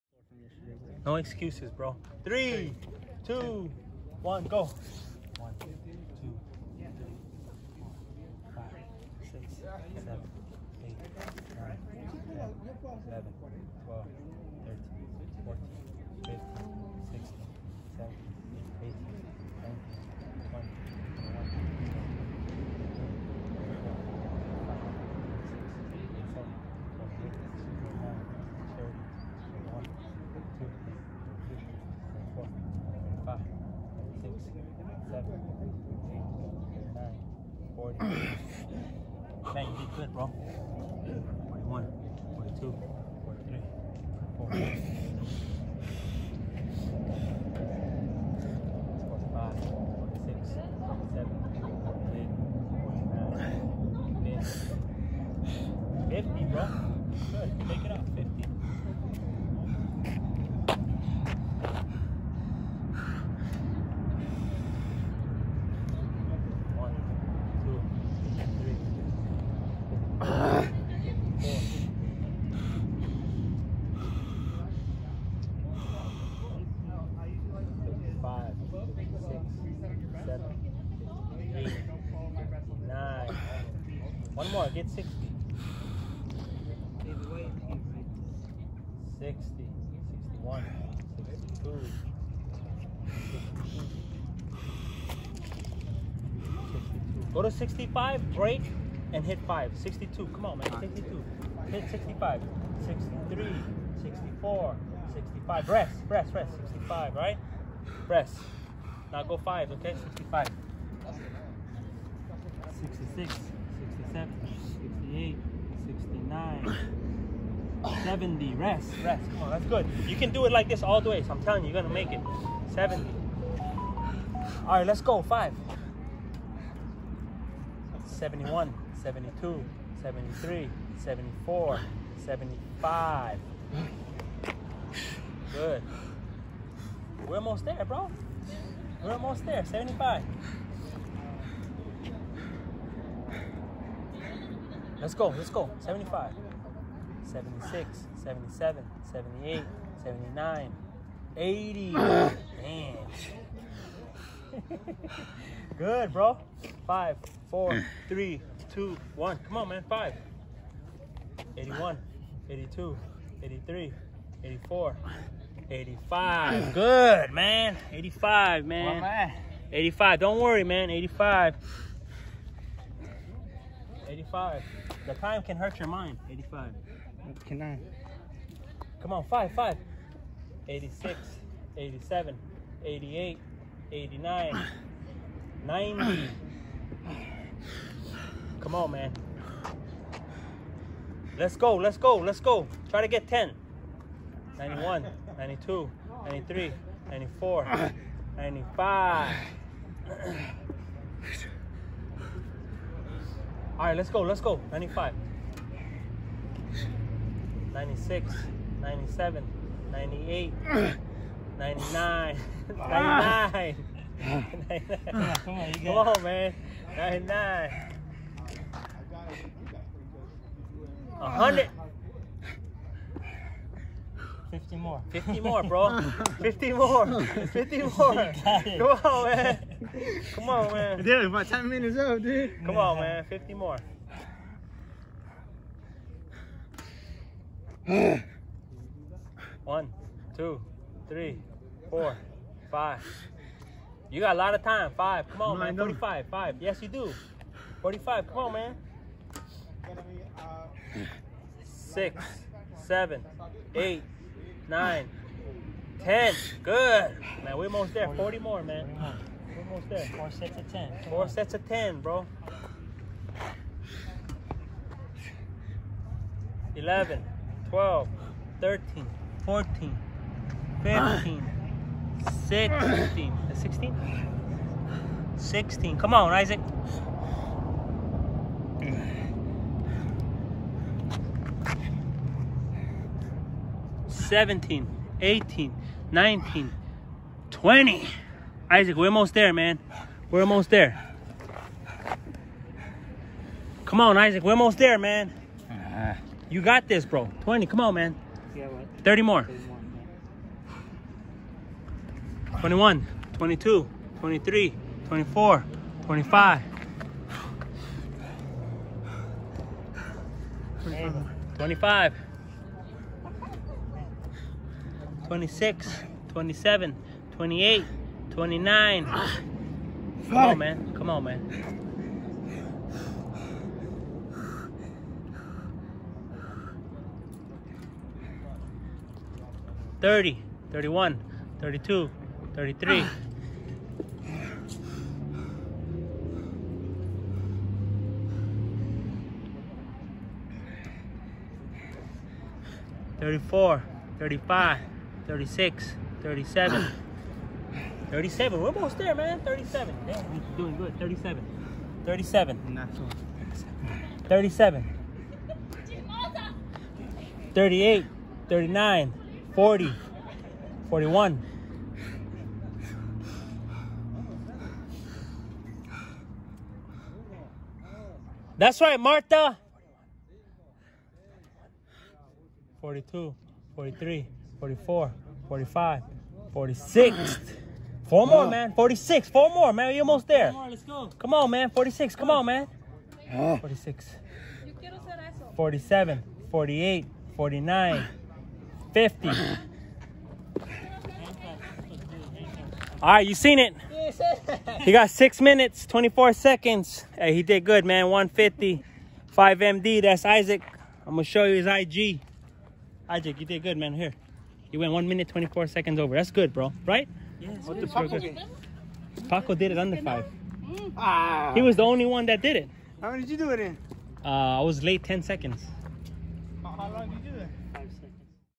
Can Isaac complete 150 push-ups in under 5 minutes?
Isaac makes an attempt to complete 150 push-ups in under 5 minutes!!

Give the routine a try for a workout!
Check him out on Instagram!!